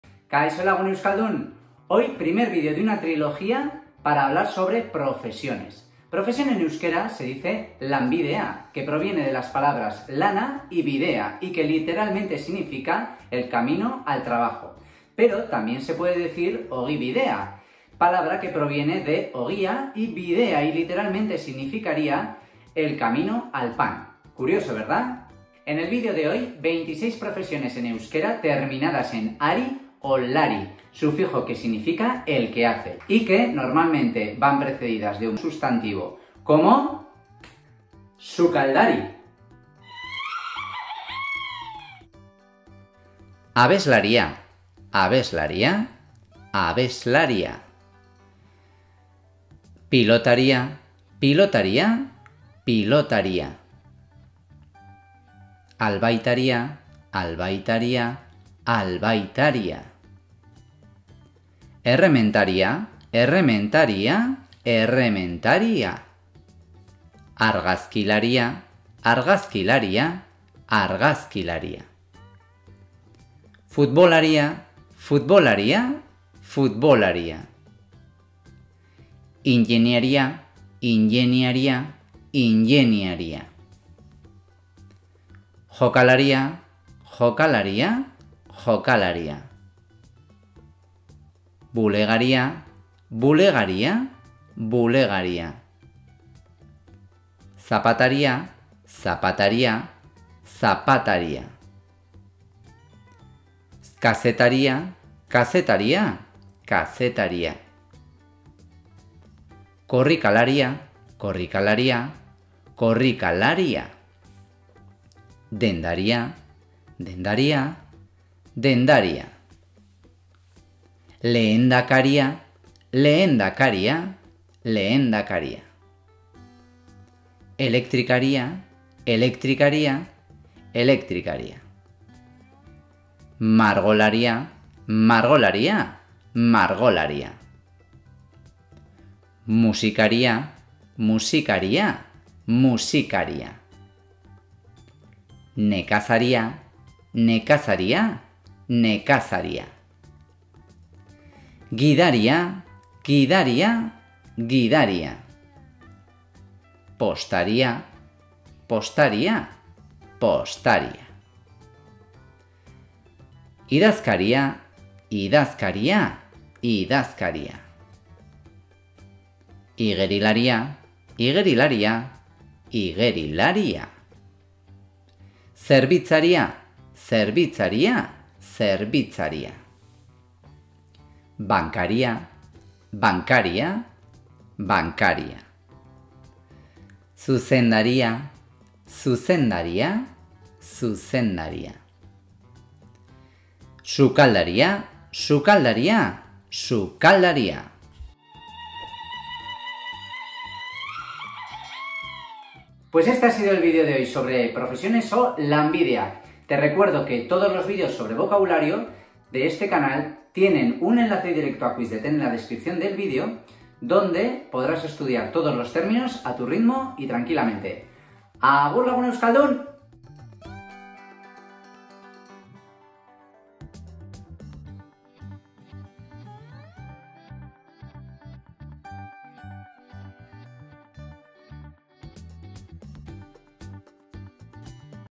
EUSKERA: PROFESIONES con sufijo -ARI. LANBIDEAK 1
Aprendemos 26 profesiones en euskera cuya terminación es -ARI o -LARI, las cuales suelen ir precedidas de un SUSTANTIVO como sukaldARI (cocinero) que procede de sukaldea (cocina), abesLARI (cantante) que procede abestia (canción) o futboLARI (futbolista) que procede de futbola.

Imágenes obtenidas de la base de recursos de Quizlet.